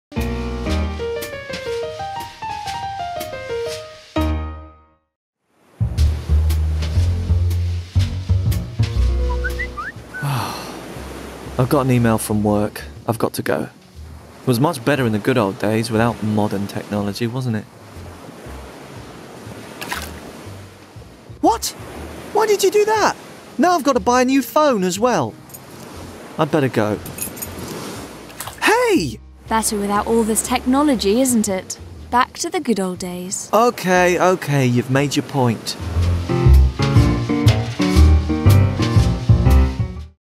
NERDS - The Good Old Days
Everybody misses the “good old days”, when technology wasn’t so invasive and we weren’t surrounded by devices, cars and so on.
But if you are so sure, you could go back to move in a carriage and send letters in order to invite your friends to see the football game at your place… on the TV you wouldn’t have.
Technology is a tool and we should always remember that every time it’s used constructively not only it makes life easier, but it helps us to build a better, more connected and more sustainable world.

The video was made in the N.E.R.D.S. (Non-formal Education Renewing Didactics in Science) Strategic Partnership project in the school sector, co-financed by the European Commission through the Erasmus+ Programme.